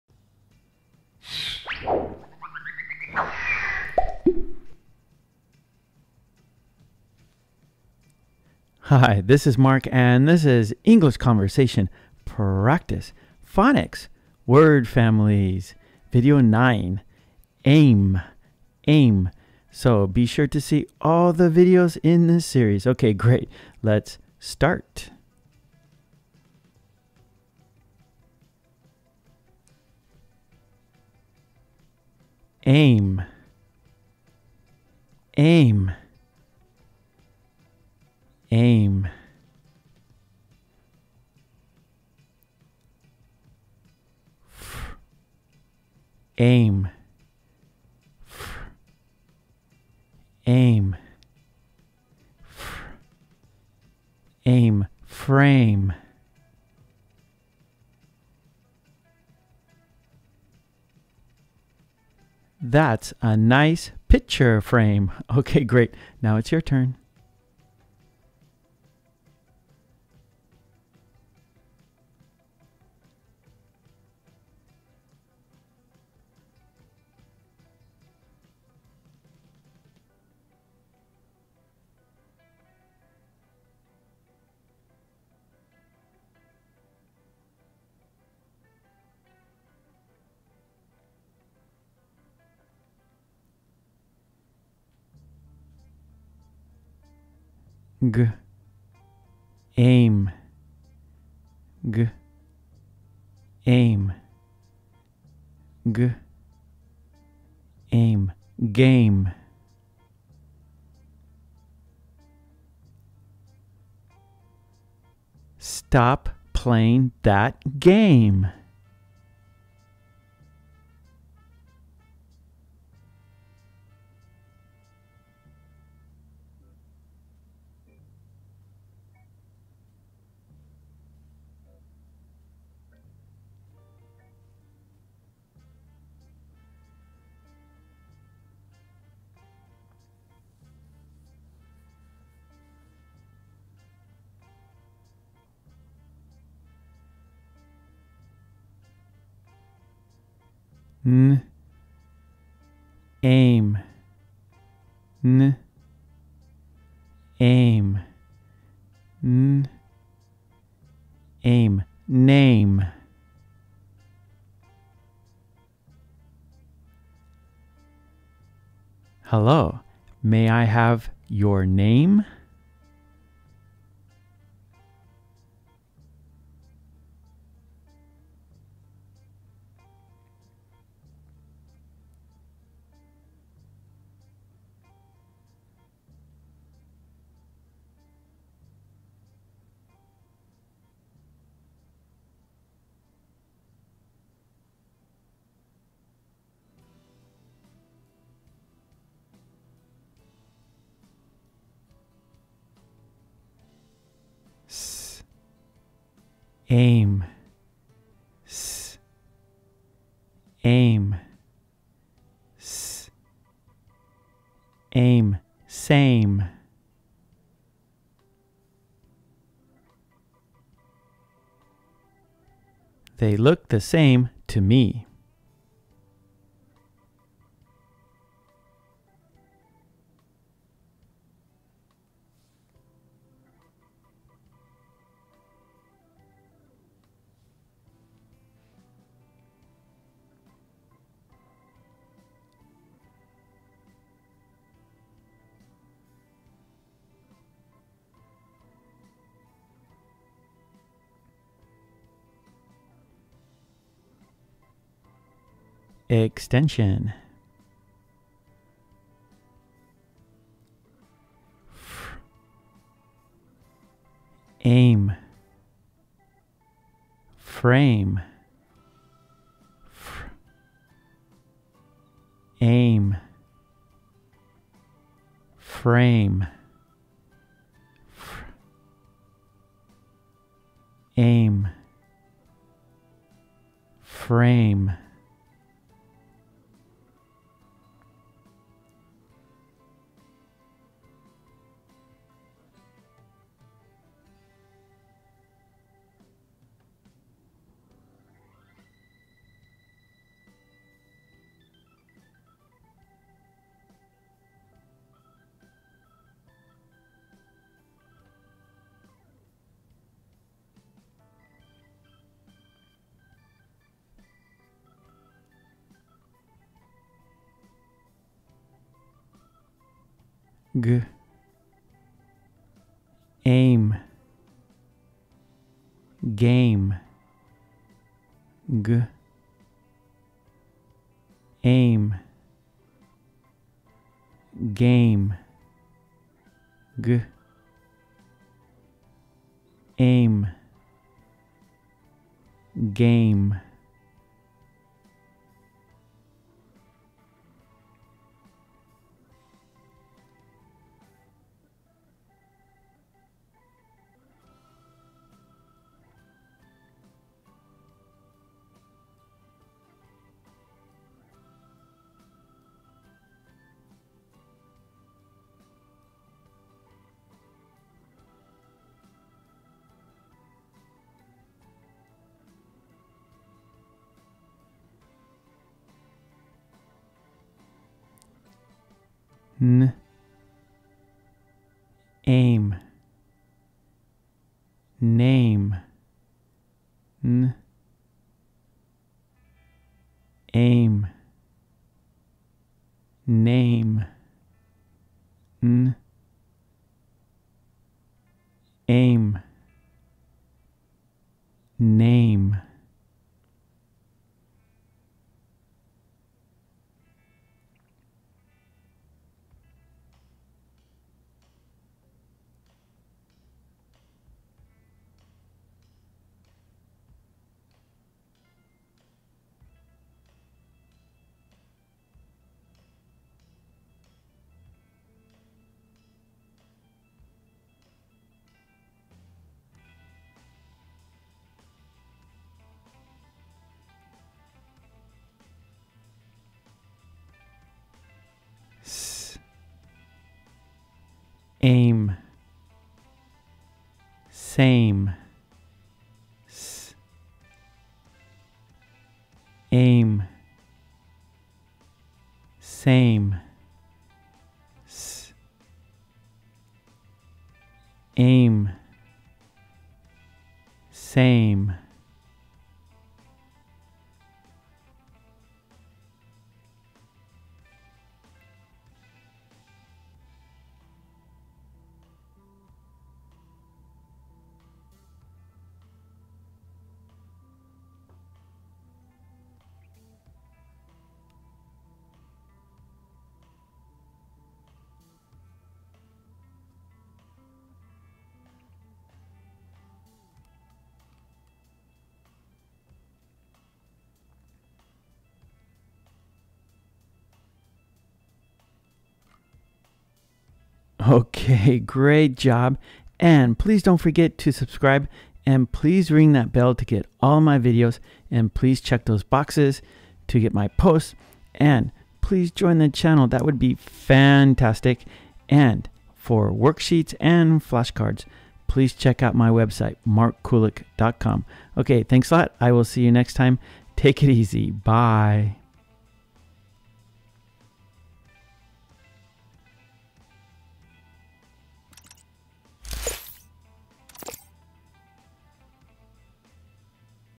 Learn Phonics - ame (word families) #9 | Mark Kulek - ESL
-ame: English Phonic Sound Combinations and Word Families for speaking English - American Pronunciation.  English Pronunciation Sound (-ame) - Learn these Phonic sounds: frame, game, name, same. Each of these phonic sounds are in sentence form to give you context and meaning. Each sentence is illustrated for better understanding. You will have many opportunities to repeat these sounds for lots of practice. In the second part of this video, you will practice on your own. This will help you with English speaking practice and build your fluency. This is an interactive listening and speaking video. Have fun learning these phonic sounds and speak English now.

English in use:
fr - ame, frame - That's a nice picture frame.
g - ame, game - Stop playing that game.
n - ame, name - Hello. May I have your name?
s - ame, same - They look the same to me.

Mark's book: 50 Very Short Conversations